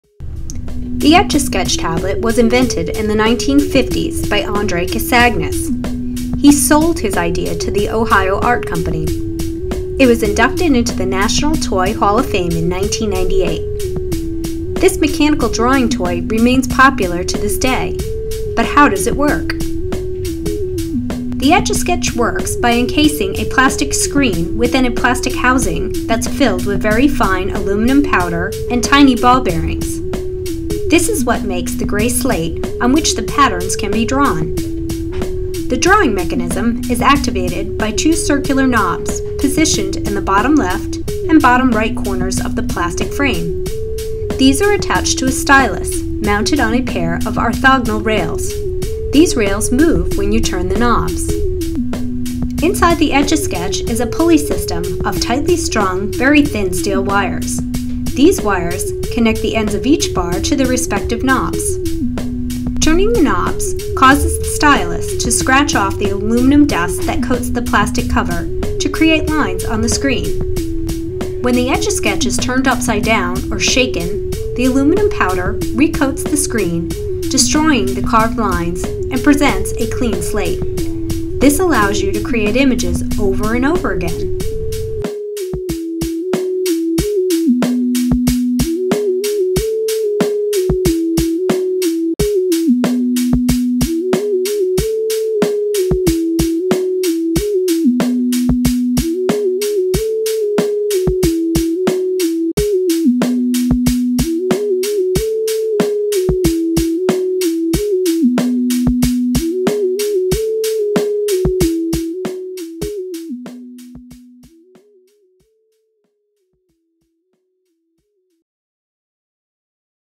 How an Etch-A-Sketch Works
I modeled the objects and did basic animation in Lightwave. I then used After Effects to tweak the animation with the Graph Editor and applied the narration.